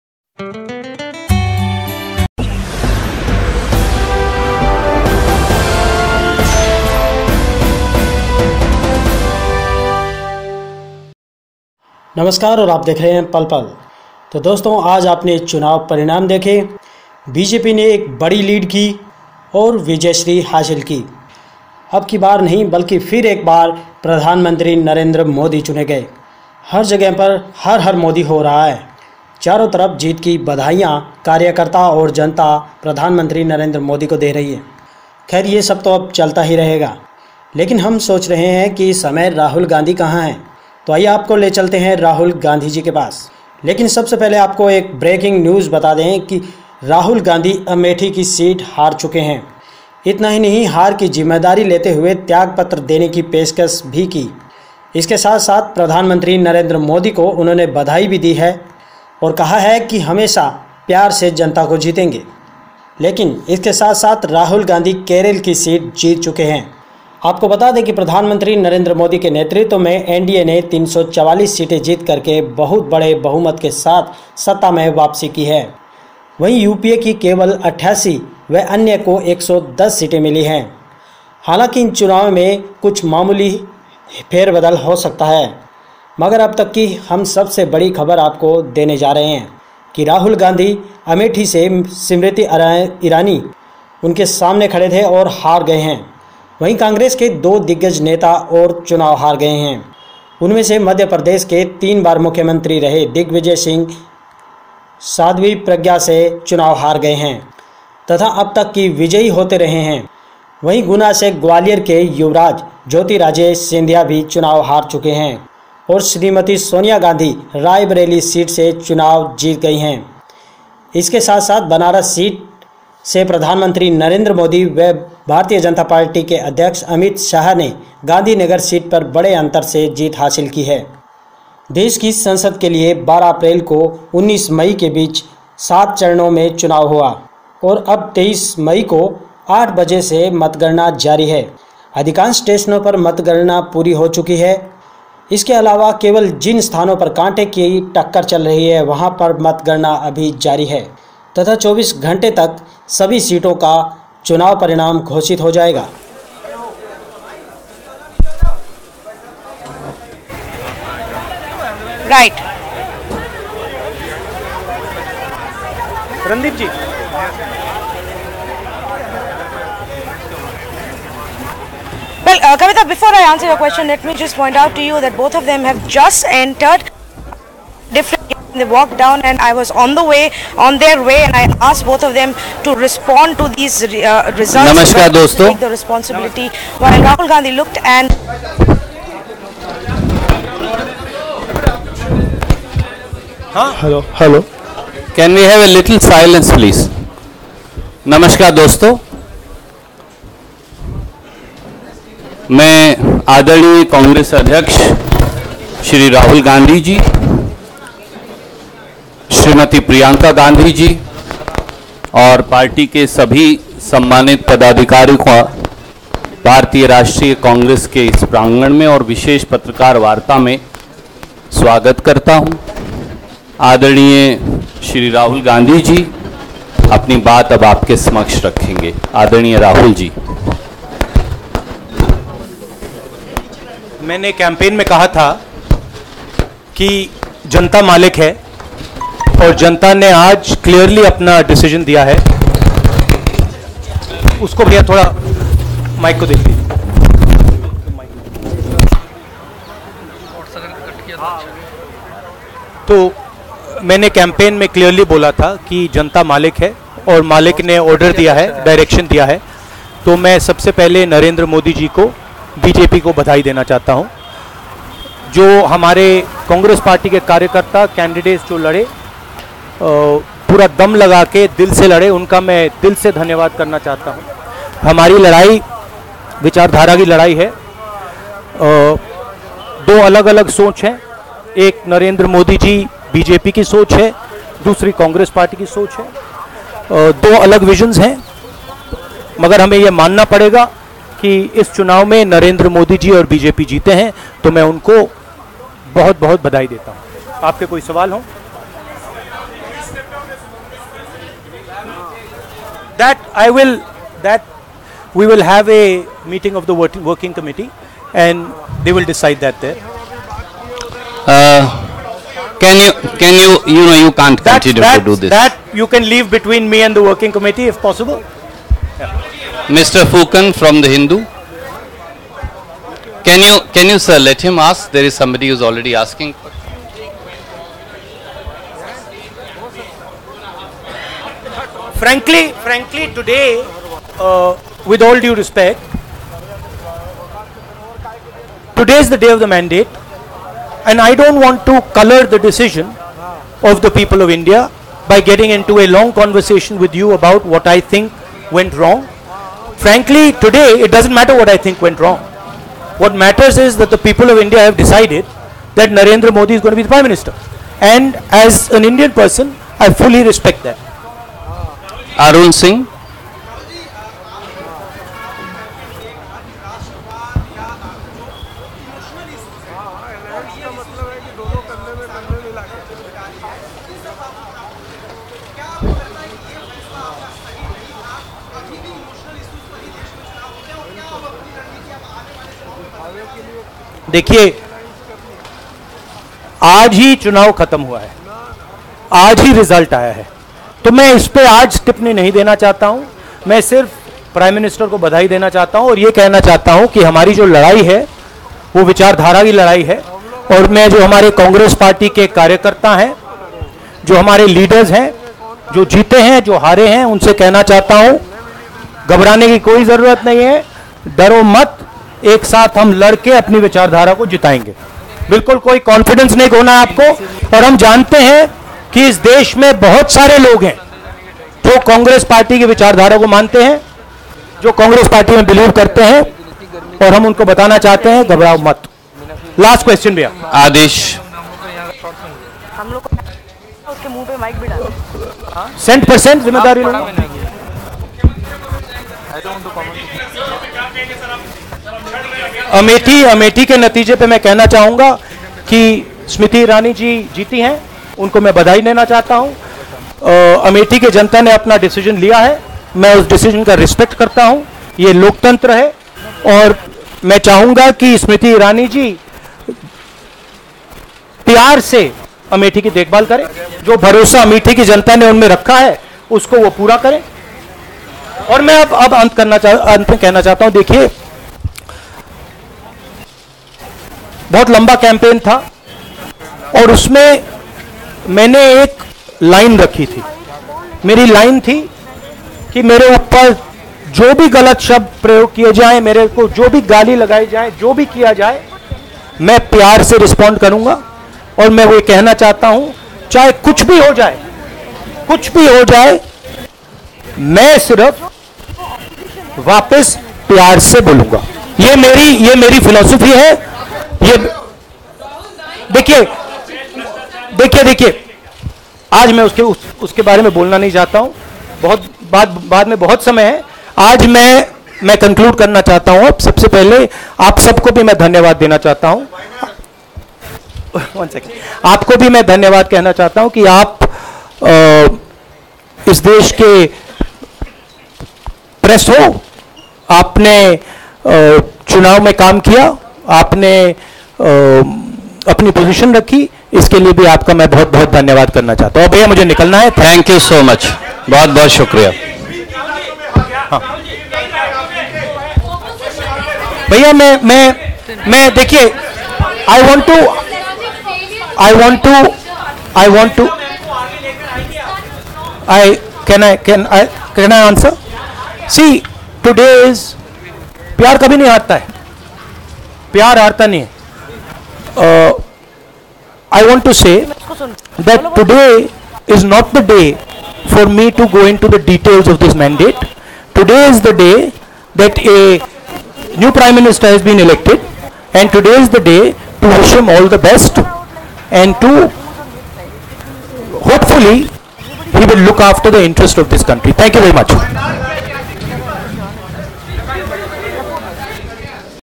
राहुलगाँधी# बोले प्यार कभी नहीं हारता-कहा हौसला रखो-rahul gandhi
Live कांग्रेसअध्यक्ष राहुलगाँधी# बोले प्यार कभी नहीं हारता-कहा हौसला रखो